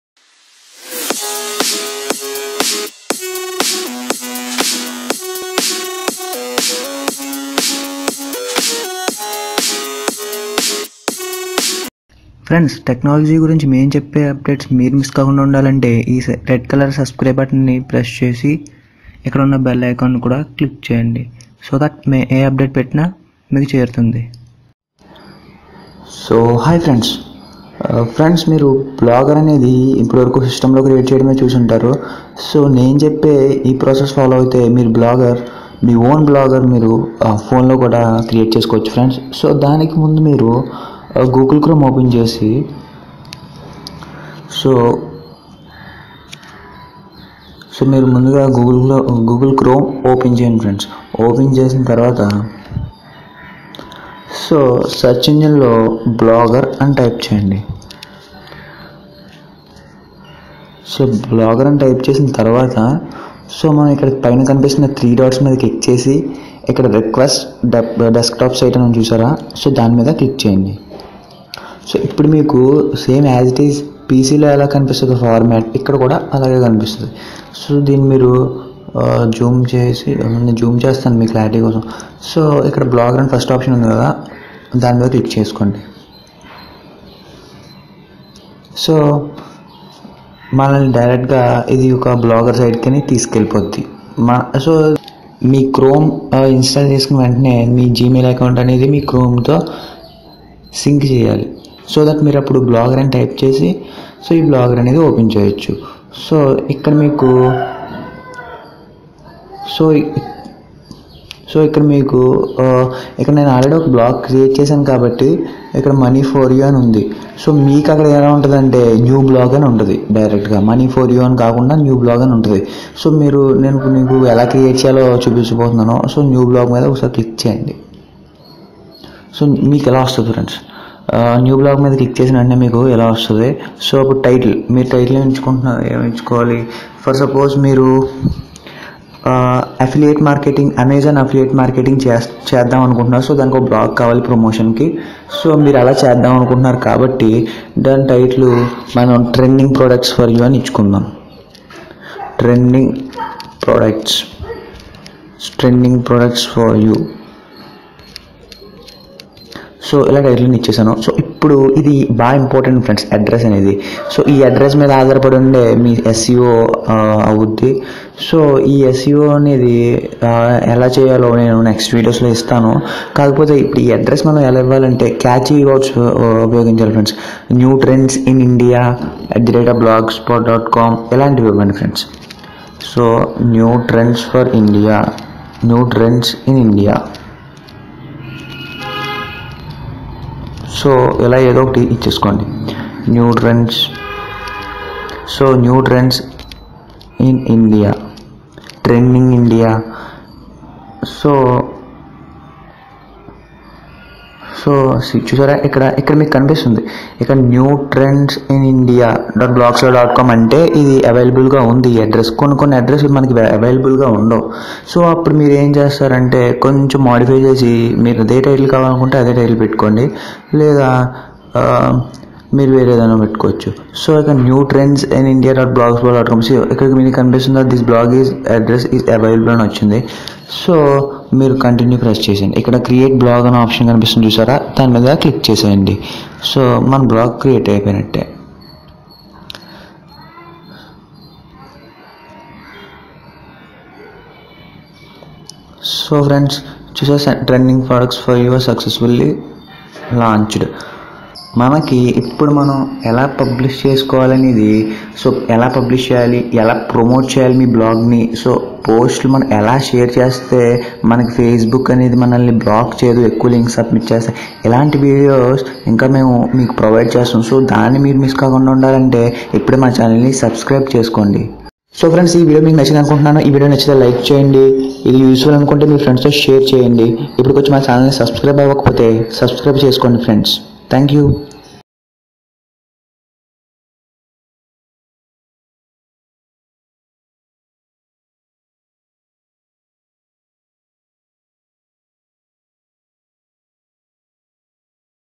how to create a blog in mobile | in telugu | blogging tutorials in telugu | telugu tech 360
In this Video I will show you how  to create a blog in mobile and some blogging tutorials in telugu.and also how to create a blog for free .creating a blog site on google is easy process in english or any other language.i will also provide blogging tutorials in telugu and course and ideas to start a blog.Business is the main reason ot start the blog.